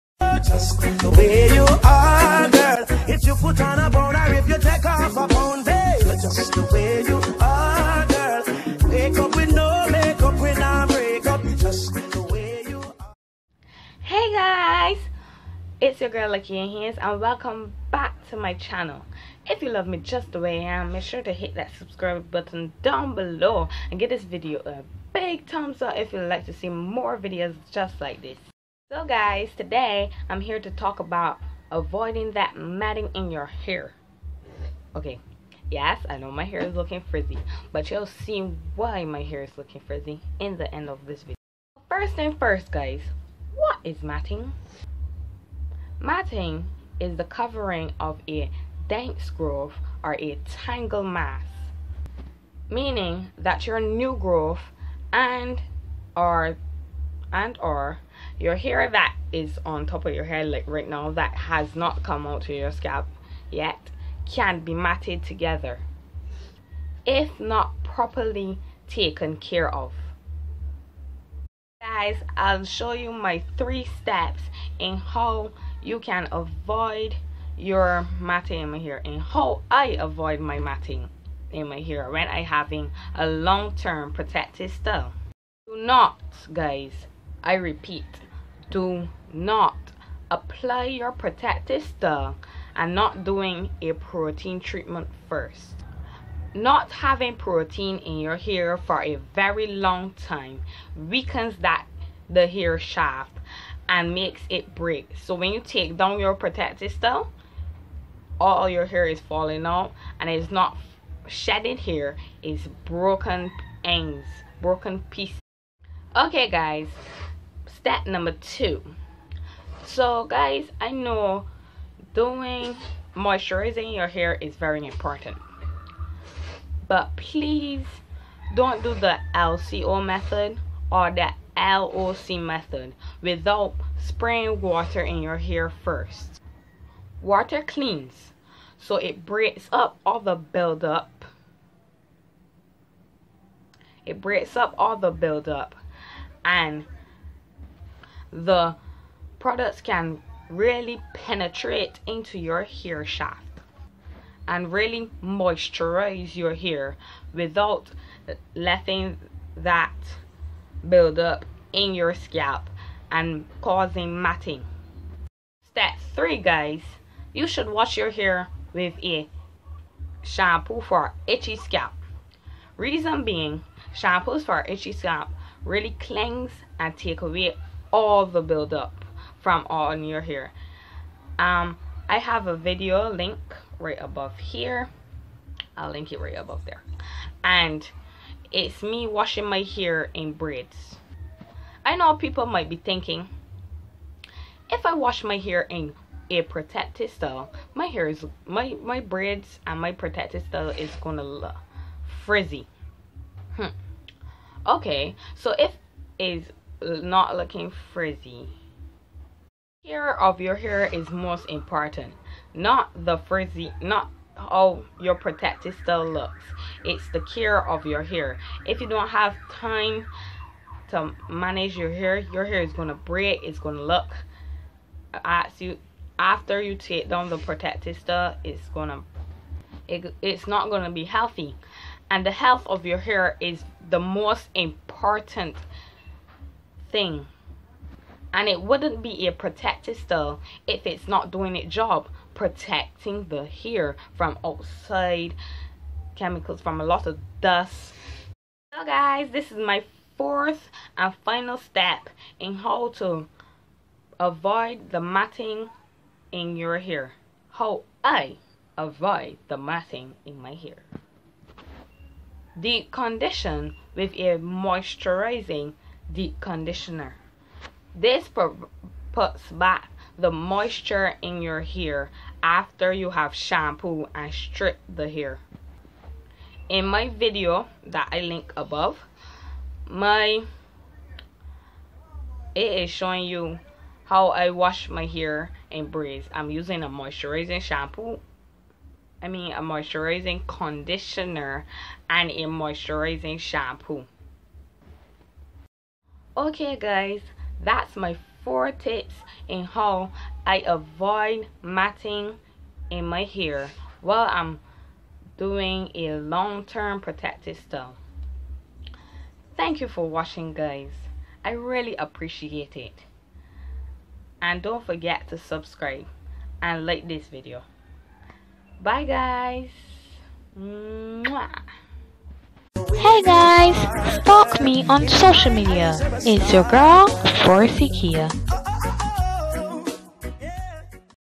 4 ways to avoid matted natural hair|talk through
Hey guys. Do you want to wear a long term protective style without it being matted together? Well watch did video for the answer💯